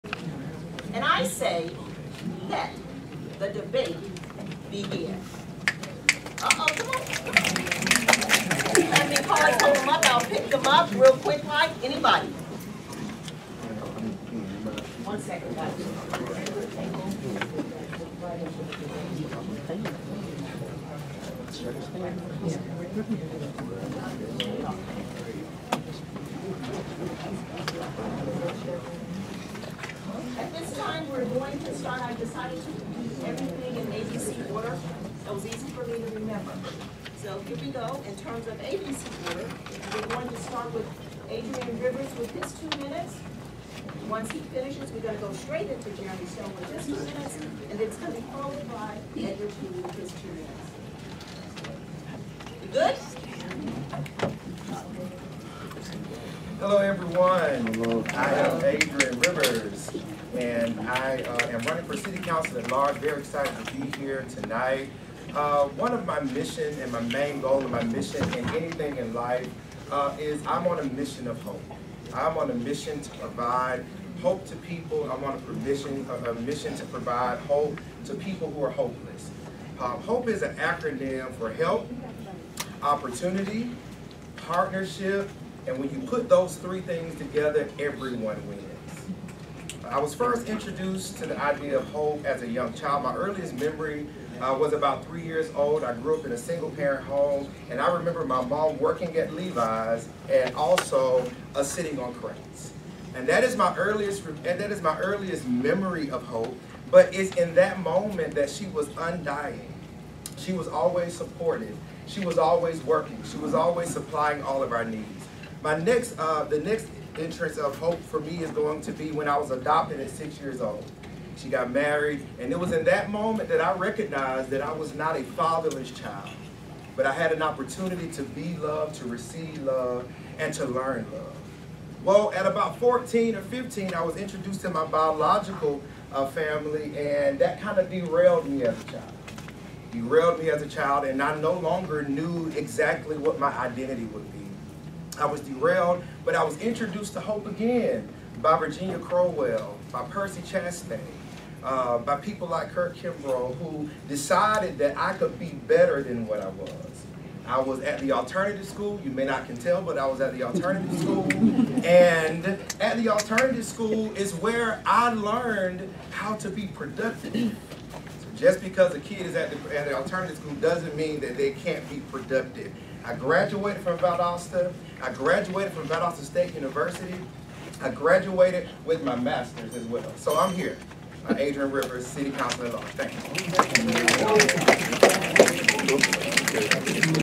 Vadosta City Council At Large - Adrian Rivers @ LCDP Candidate Debate
Lowndes County Democratic Party General Meeting - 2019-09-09
Candidate Debate